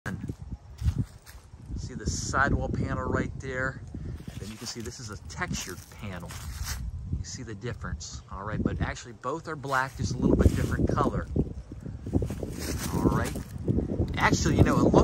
“Black Metal”…changing the barn…industry in 2023 🐾
32x64 pole barn building under construction.